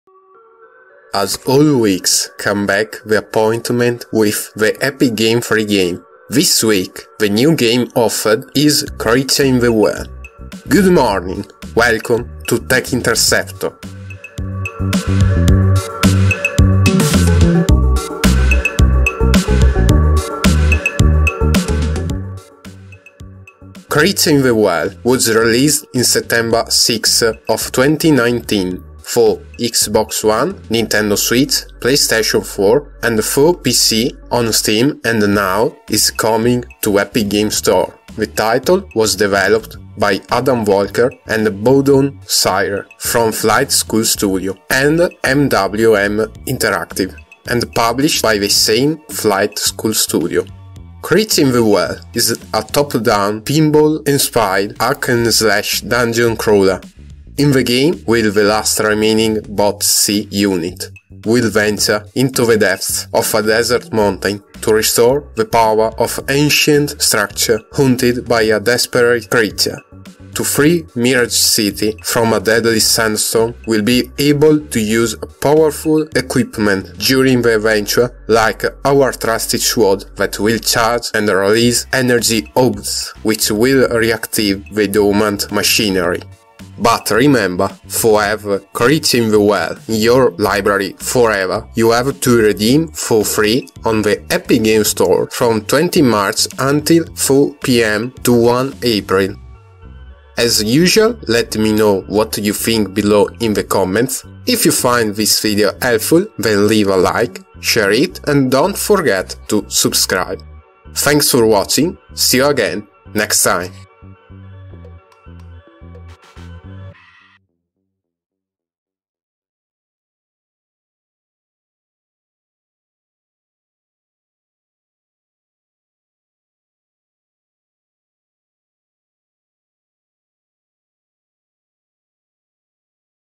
Creature in the Well is the new Epic Games Store free game!
As all weeks come back the appointment with the Epic Game free game.
This week the new game offered is Creature in the Well.
Creature in the Well was released in September 6 of 2019 for Xbox One, Nintendo Swich, Play Station 4 and for PC on Steam and now is coming to Epic Games Store.
The title was developed by Adam Volker and Bohdon Sayre from Flight School Studio and MWM Interactive, and published by the same Flight School Studio.
Creature in the Well is a top-down, pinball-inspired, hack-and-slash dungeon crawler.
In the game we'll the last remaining BOT-C unit. We'll venture into the depths of a desert mountain to restore the power of an ancient structure, haunted by a desperate creature. To free the city of Mirage from a deadly sandstorm, we will be able to use powerful equipment during the adventure, like our trusty sword that will charge and release energy orbs, which will reactivate the dormant machinery.
But remember, for have Creature in the Well in your library forever you have to redeemed for free on the Epic Games Store from 25 March until 4 pm to 1 April.

Windows System Requirements

Minimum
OS: Windows 7 (64 bit)
CPU: Intel Core i3-530
Graphics card: NVIDIA GeForce GTX 470
Memory: 4 GB
Storage: 5 GB

Recommended
OS: Windows 10 (64 bit)
CPU: Intel Core i5
Graphics card: NVIDIA GeForce GTX 760
Memory: 8 GB
Storage: 5 GB